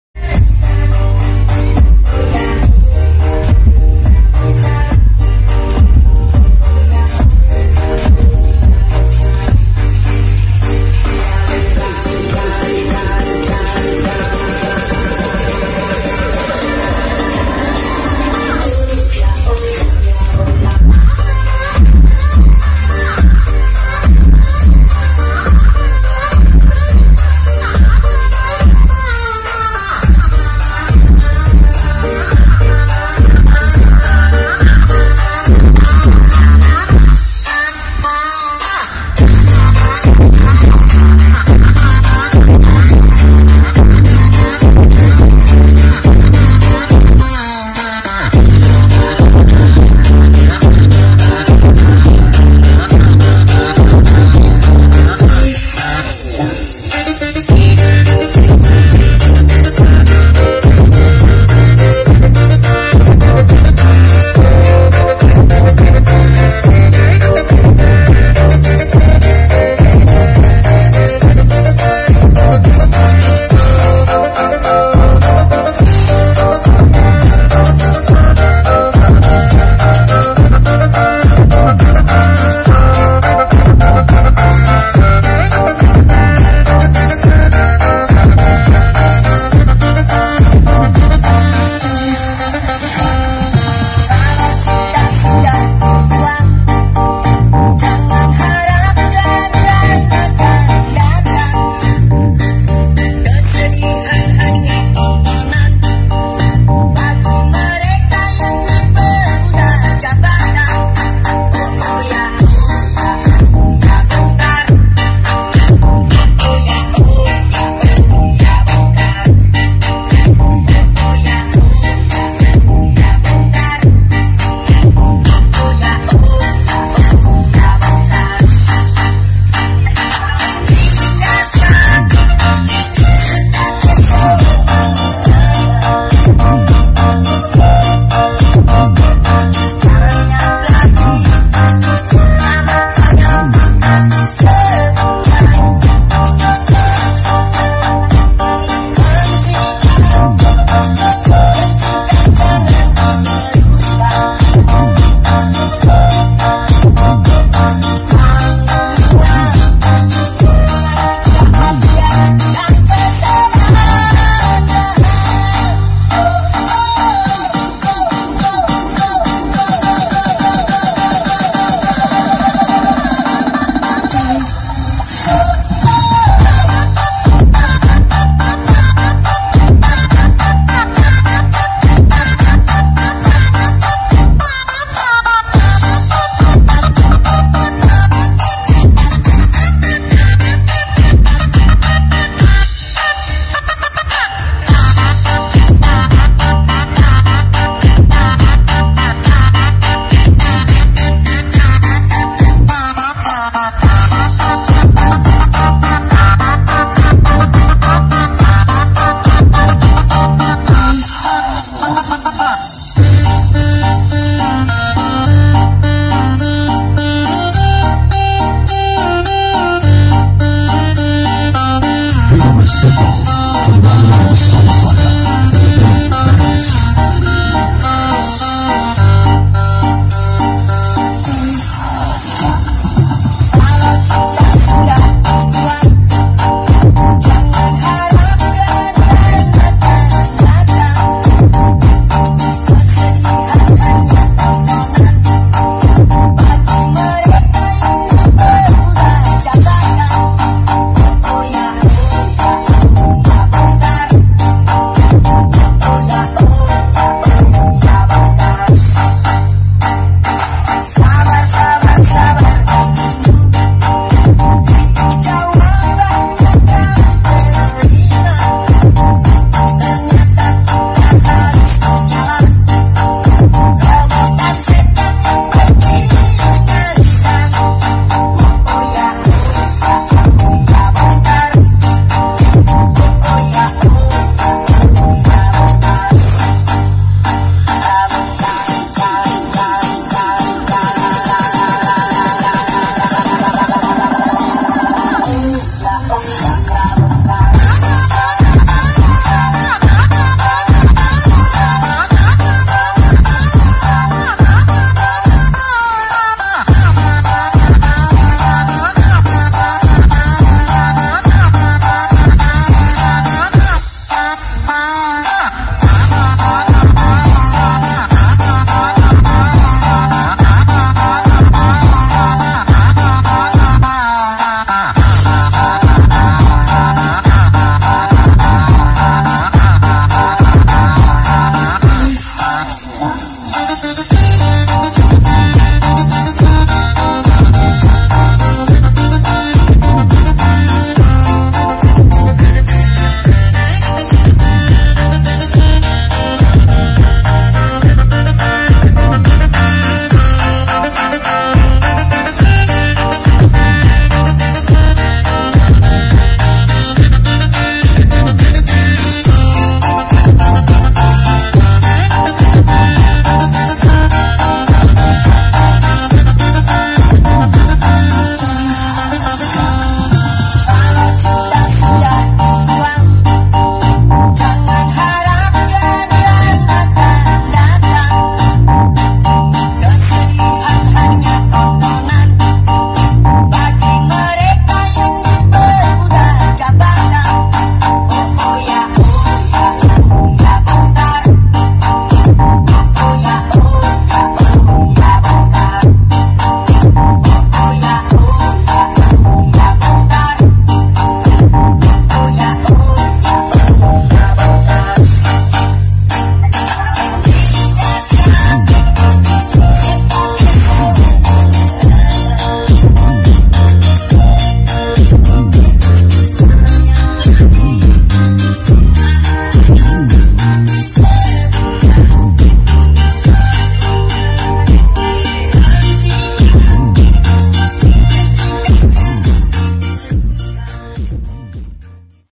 IDOLA Sound Tampil Mantab Di Karnaval Petungroto Kec, Ngajum
Idola sound system tampil maxsimal di karnaval petungroto kec, ngajum . Dengan pasukan karnaval yang kompak .
Acara memperingati hari kemerdekaan indonesia.
Di laksanakan pada tanggal 01 september 2019

Orisinal video Oke BosKu